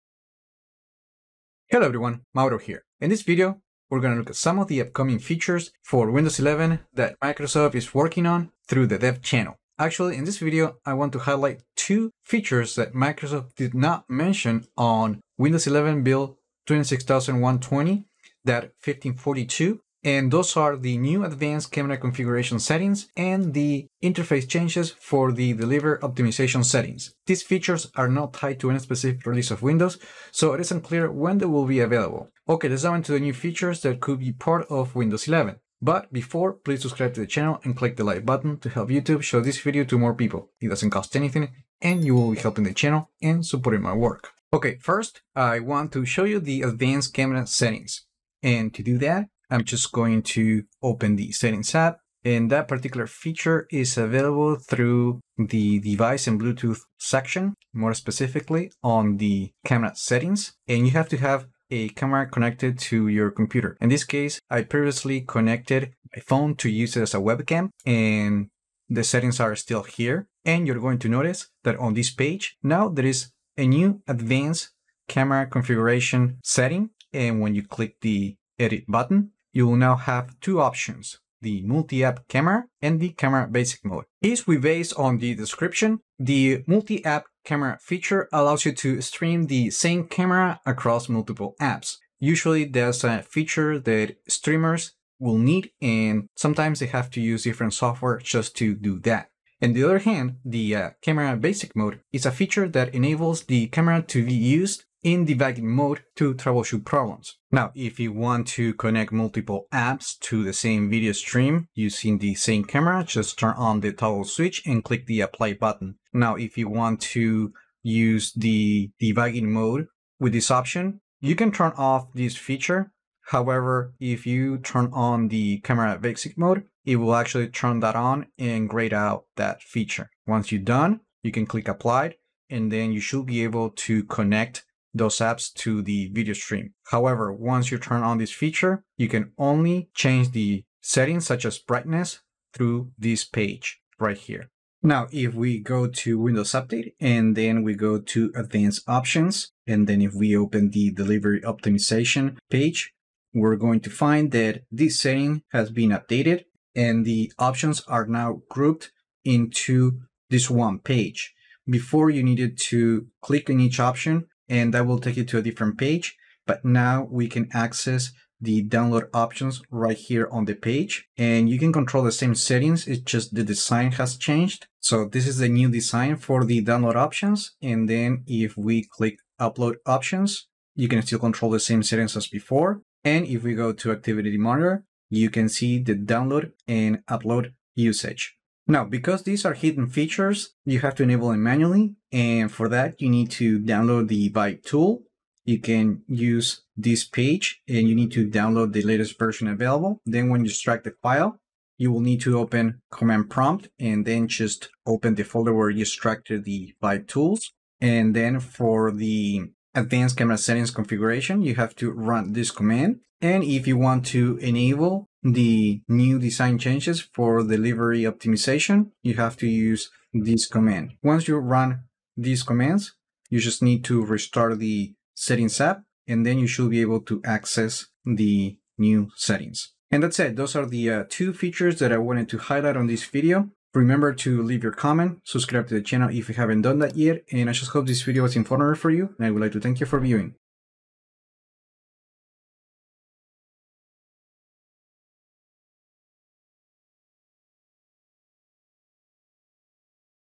Windows 11: NEW Multi-app Camera and Delivery Optimization redesign (build 26120.1542)
Windows 11 hands-on video with multi-app camera feature and Delivery Optimization device changes. Follow ➡️ link for full steps ✅
In this video, I will walk you through the steps to the new Multi-app camera feature to stream the same camera to multiple apps without workarounds and the new design change for the Delivery Optimization settings available on Windows 11 build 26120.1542 (KB5041872).

00:00 Intro
00:31 Multi-app camera feature
02:25 Deliver Optimization UI update
03:08 Enable hidden features via ViveTool
03:50 Closing